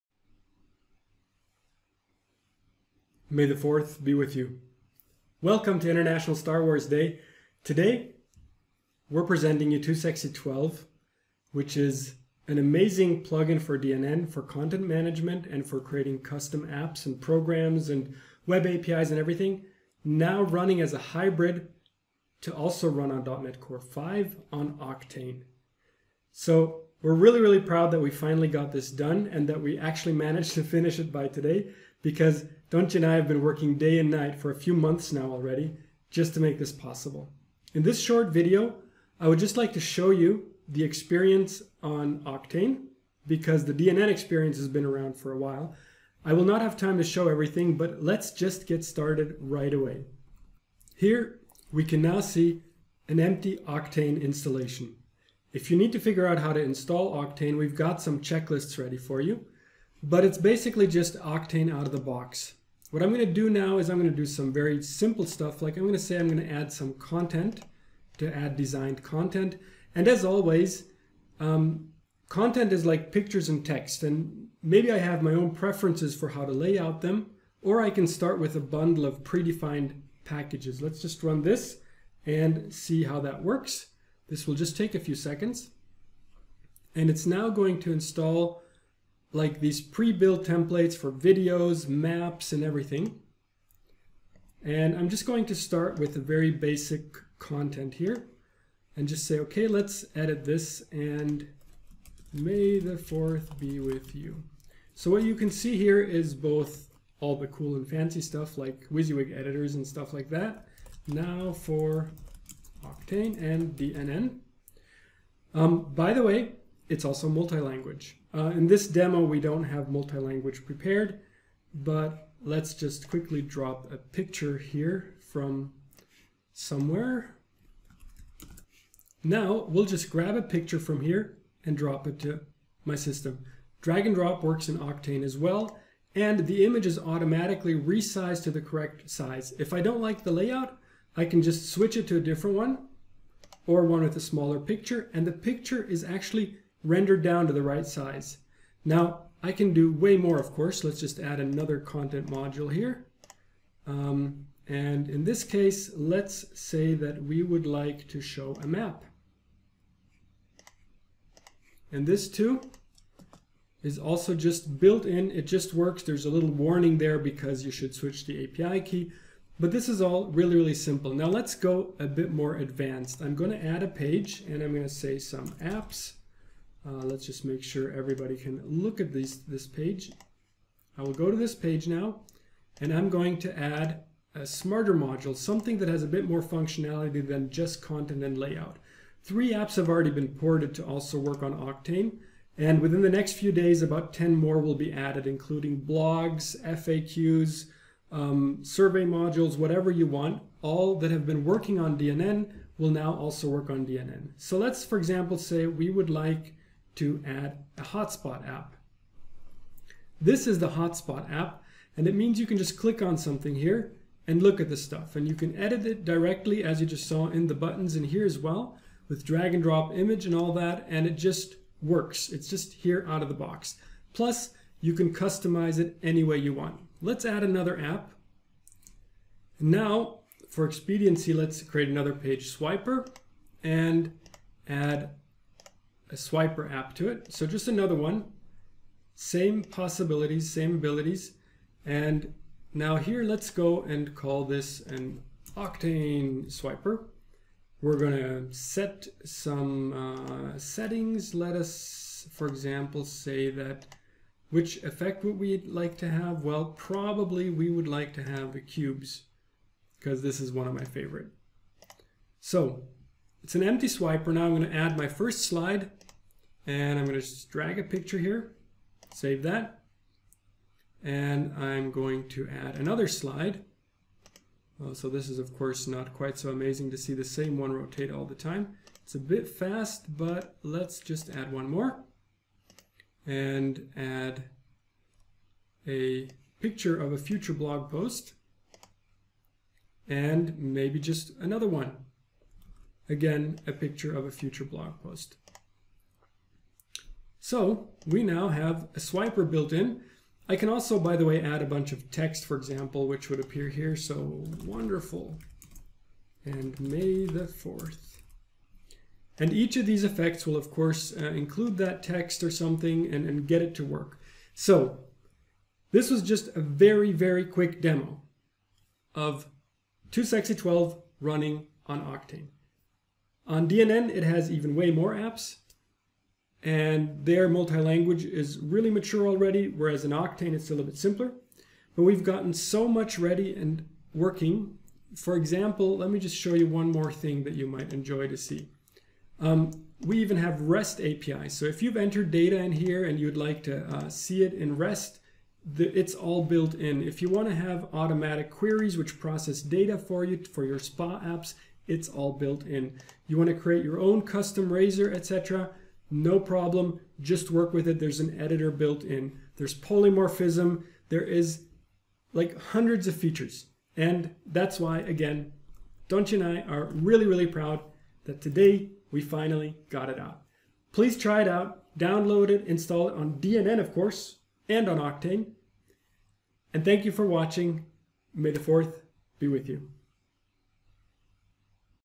2sxc 12 Hybrid Released for Dnn and Oqtane - May the 4th...
Release video of 2sxc 12 which is a Hybrid CMS and App platform which runs on Dnn (.net classic) and Oqtane (.net core / Blazor)